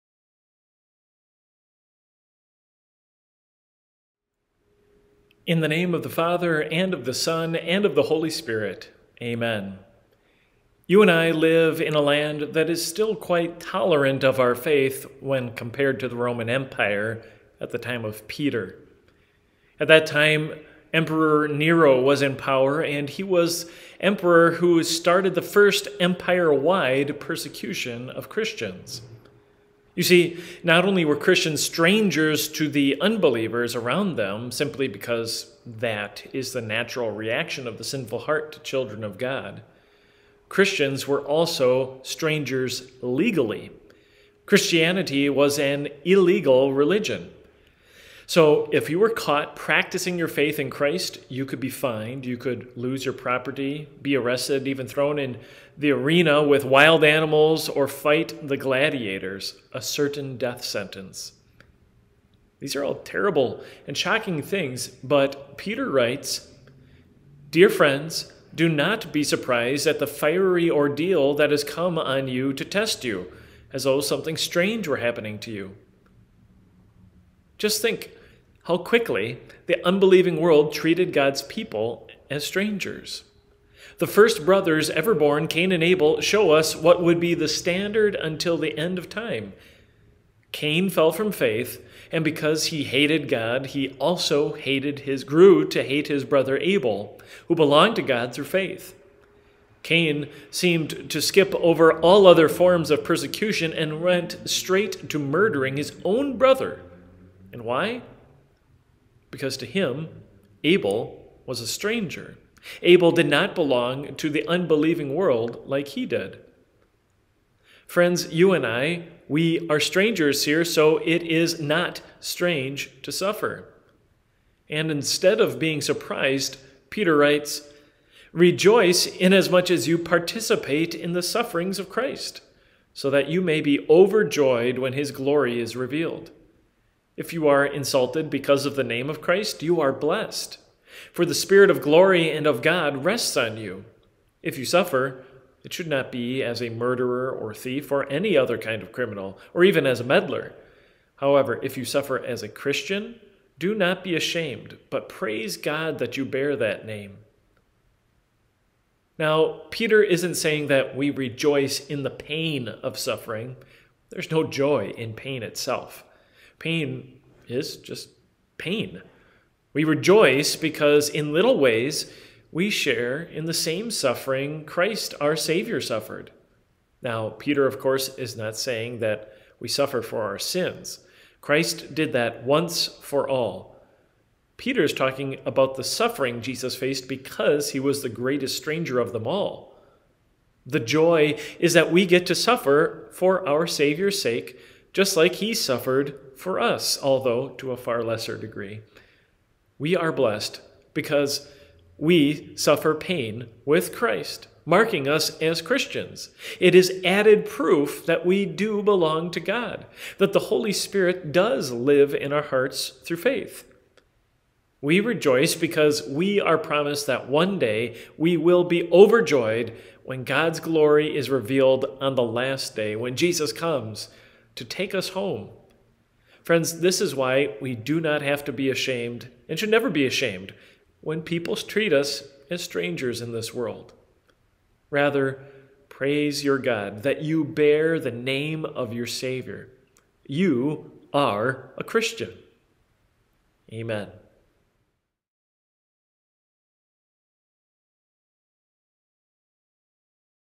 Not Strange to Suffer - A Devotion on 1 Peter 4:12-16 from Pastor Ben Enstad
Nobody wants to suffer, but it is the natural condition of the believer during their lives on earth.